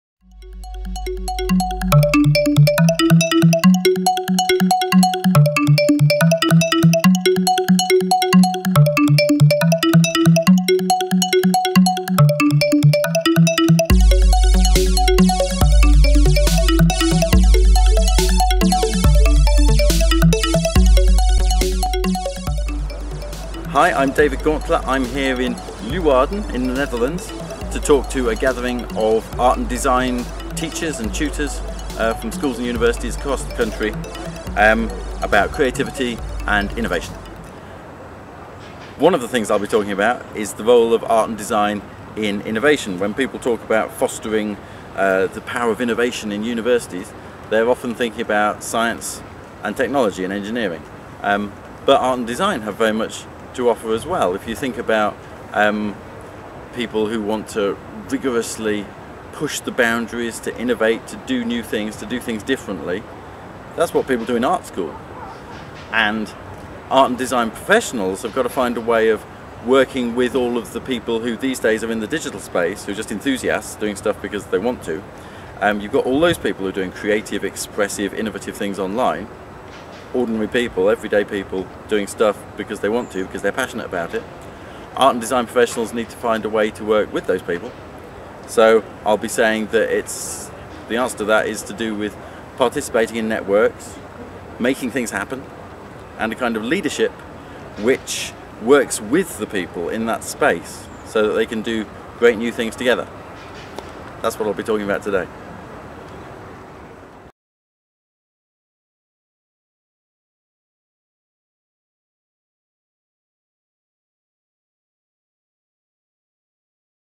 Art and design - and innovation
David Gauntlett talks about the role of art and design in innovation. Recorded at Skills 21 Kunsten in Leeuwarden, the Netherlands, in October 2012.